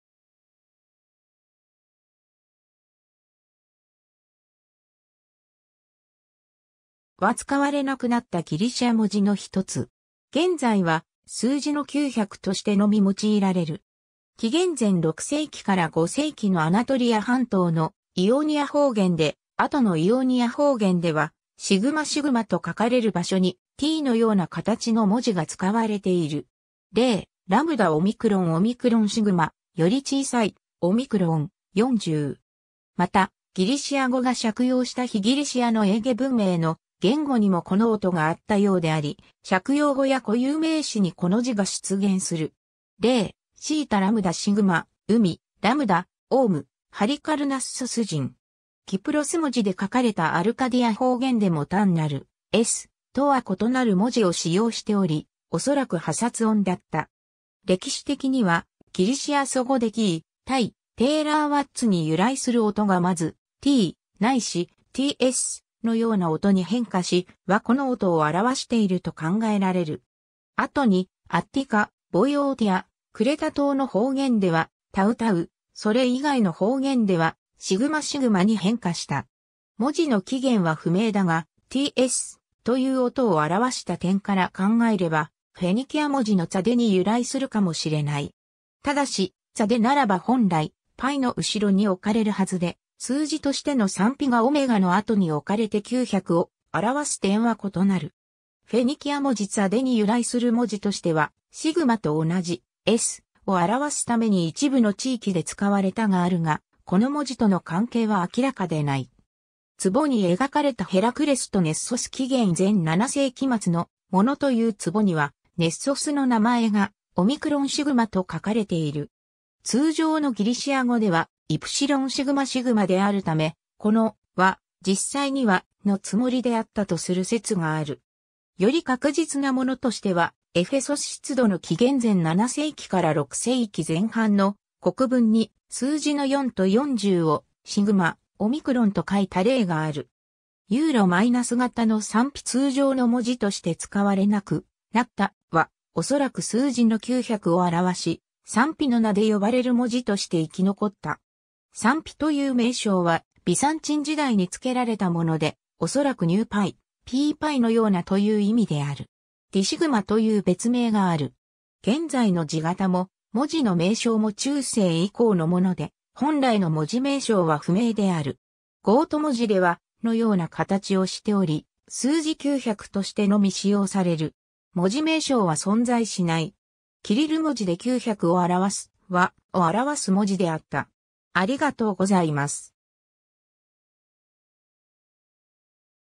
Ϡ
Ϡ, ϡ（サンピ、ギリシア語: σαμπί）は使われなくなったギリシア文字の一つ。
現在は数字の900としてのみ用いられる。
 紀元前6世紀から5世紀のアナトリア半島のイオニア方言で、後のイオニア方言ではσσ(ss)と書かれる場所にTのような形の文字（Ͳ）が使われている。
例: ΕΛΑͲΟΝΟΣ (ἐλάσσονος)「より小さい（属格）」、 ΤΕͲΑΡΑϘΟΝΤΑ (τεσσαράκοντα)「40」。
また、ギリシア語が借用した非ギリシアのエーゲ文明の言語にもこの音があったようであり、借用語や固有名詞にこの字が出現する。
例: ΘΑΛΑͲΗΣ (θαλάσσης)「海（属格）」、ΑΛΙΚΑΡΝΑͲΕΩΝ (Ἁλικαρνασσέων)「ハリカルナッソス人（複数属格）」。
 キュプロス文字で書かれたアルカディア方言でも単なる/s/とは異なる文字を使用しており、おそらく破擦音だった。
 歴史的にはギリシア祖語で *k(ʷ)(ʰ)y, *t(ʰ)y, *tw に由来する音がまず/tʃ/ないし/ts/のような音に変化し、「Ͳ」はこの音を表していると考えられる。
後にアッティカ、ボイオーティア、クレタ島の方言では「ττ」(tt)、それ以外の方言では「σσ」(ss)に変化した。
 文字の起源は不明だが、/ts/という音を表した点から考えれば、フェニキア文字のツァデに由来するかもしれない。
ただし、ツァデならば本来「Π」の後ろに置かれるはずで、数字としてのサンピがΩの後に置かれて900を表す点は異なる。
フェニキア文字ツァデに由来する文字としては、Σ（シグマ）と同じ/s/を表すために一部の地域で使われたϺ（サン）があるが、この文字との関係は明らかでない。
 壺に描かれたヘラクレスとネッソス 紀元前7世紀末のものという壺にはネッソスの名前が「ΝΕΤΟΣ」と書かれている。
通常のギリシア語では「Νεσσος」であるため、この「Τ」は実際には Ͳ のつもりであったとする説がある。
 より確実なものとしては、エフェソス出土の紀元前7世紀から6世紀前半の刻文に数字の4と40を「ΤΕͲΑΡΕΣ」(=τέσσαρες)、「ΤΕͲΑΡΑϘΟΝΤΑ」(=τεσσαράκοντα) と書いた例がある。
 €-型のサンピ 通常の文字として使われなくなった「Ͳ」は、おそらく数字の900を表し「サンピ」の名で呼ばれる文字(ϡ)として生きのこった。
 サンピという名称はビザンチン時代につけられたもので、おそらく ὧς ἂν πῖ 「ピーΠのような」という意味である。
「ディシグマ」という別名がある。
現在の字形も文字の名称も中世以降のもので、本来の文字名称は不明である。
 ゴート文字では「↑」のような形をしており、数字900としてのみ使用される。
文字名称は存在しない。
キリル文字で900を表す「Ѧ」は、[ẽ]を表す文字であった。